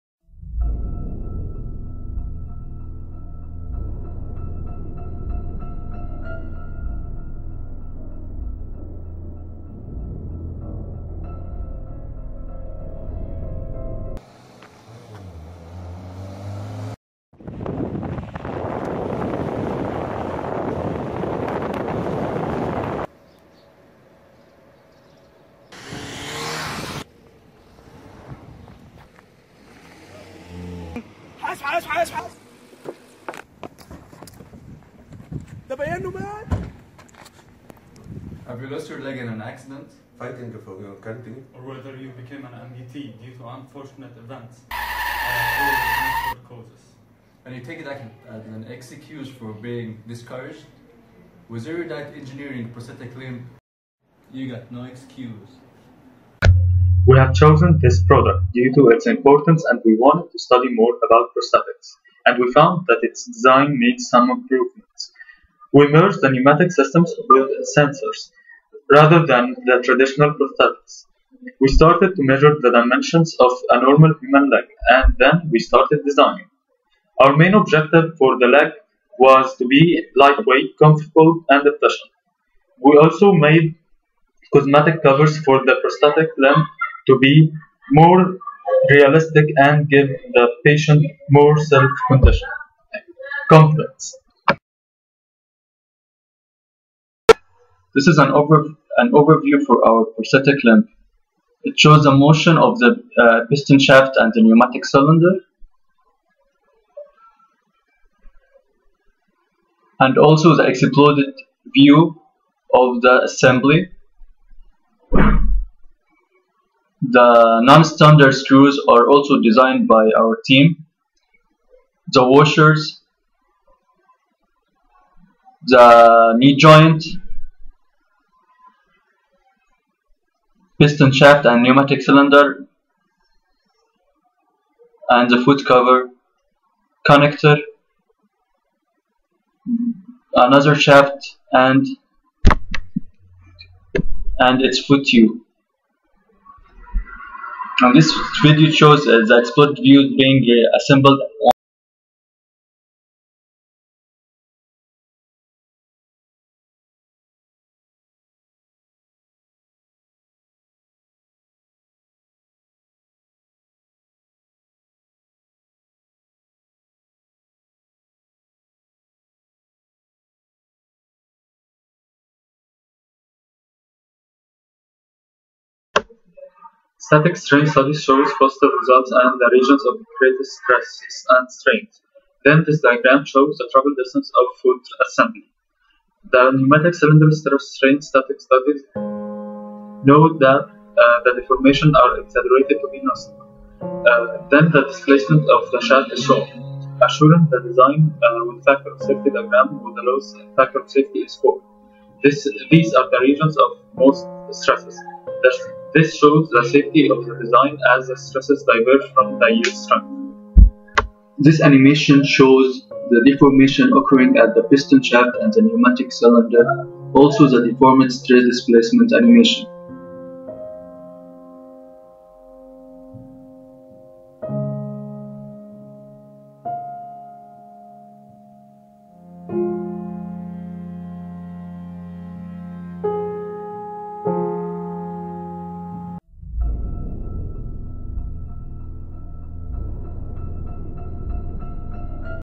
BUE ME SolidWorks Projects - Prosthetic Leg
BUE Mechanical Engineering Year 1 Student SolidWorks Projects - Prosthetic Leg